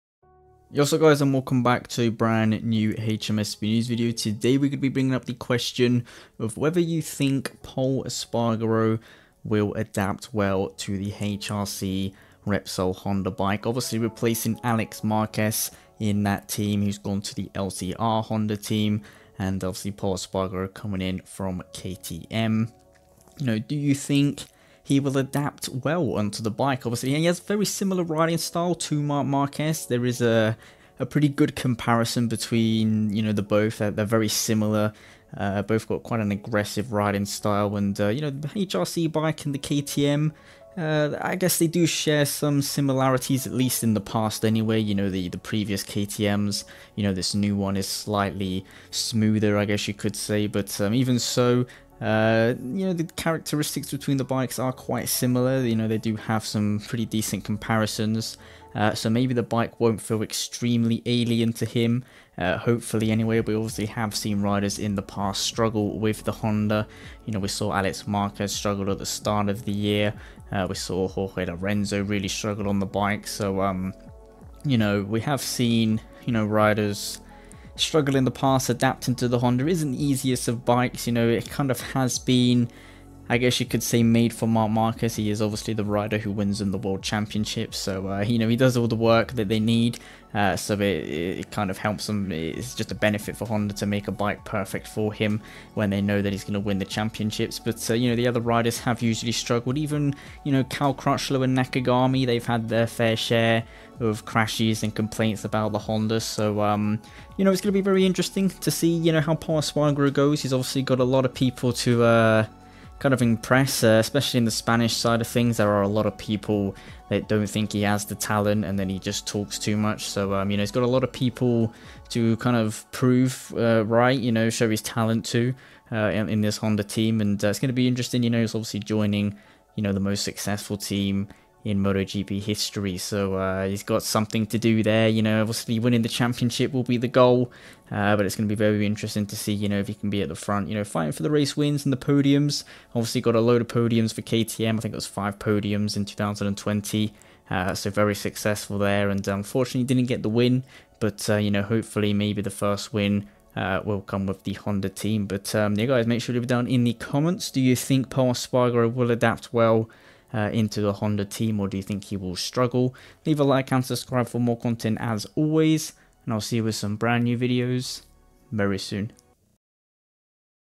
WILL POL ESPARGARO ADAPT TO THE HRC HONDA?! - MotoGP News
Do you think we will see Pol adapt quickly to his new Honda machine?
Social Media!
My personal channel!